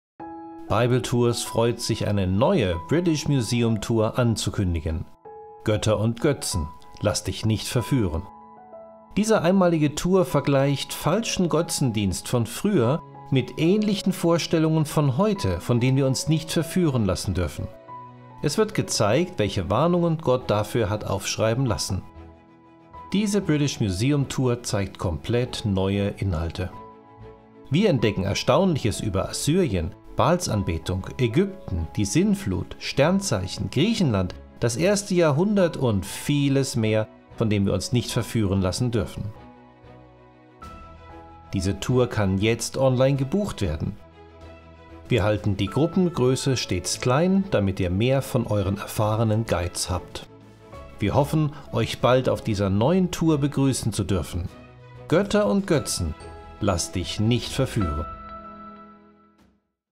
Götter & Götzen – Lass Dich Nicht Verführen! | VIRTUELLE BIBEL TOUR | DEUTSCH
In dieser NEUEN Tour besprechen wir in einer Stunde viele Hinweise auf falsche Götter und Götzen aus alter Zeit, von denen wir uns auch heute nicht verführen lassen dürfen.

Wie hat Gott vor falscher Anbetung gewarnt?
Welche Verbindung besteht zwischen der Sintflut und heutigen Festen?
Was lehren uns Anbetungsformen aus Griechenland, Ägypten und Assyrien?
Sehe, was die Archäologie über Sternzeichen, Baal, Ischtar und Maria lehrt
Beachte Warnungen aus der Bibel
Exklusive Bilder und ein Rundgang im British Museum über Google Maps
Verbringe Zeit mit der Bruderschaft (virtuell)

Diese Tour enthält völlig neue Inhalte und Objekte, die wir schon früher verwendet haben. Wir freuen uns, die zu zeigen, von welchen Göttern und Götzen wir uns nicht verführen lassen sollen.

Wir verwenden 3D-Bilder, Karten, Künstlerimpressionen und einen einzigartigen Rundgang durch das Britische Museum, so dass es sich anfühlt, als würdest du das Museum persönlich mit einem unserer erfahrenen Guides besuchen. Außerdem halten wir die Gruppengröße überschaubar, damit du einen Anteil an der Tour haben kannst.